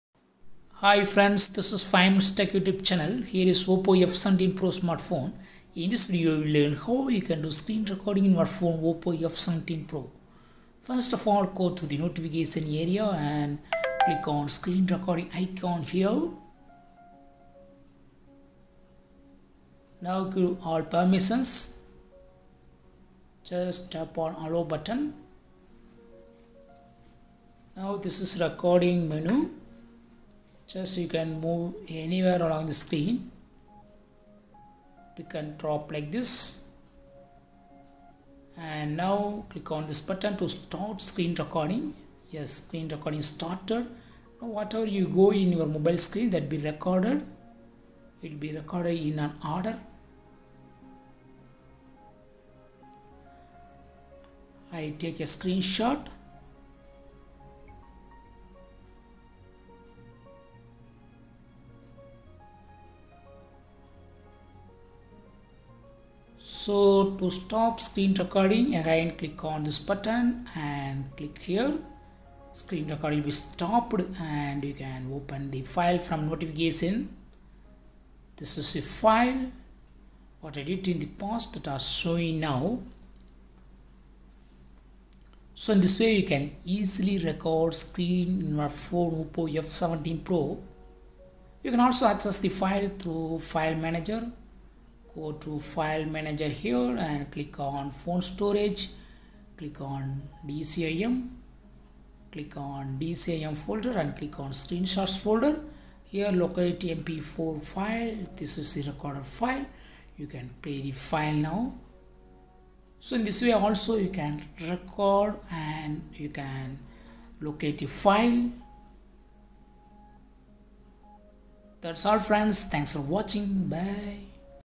How to do screen recording in Oppo F17 Pro
Oppo F17 Pro screen recording settings: Learn here how to do screen recording in Oppo F17 Pro smartphone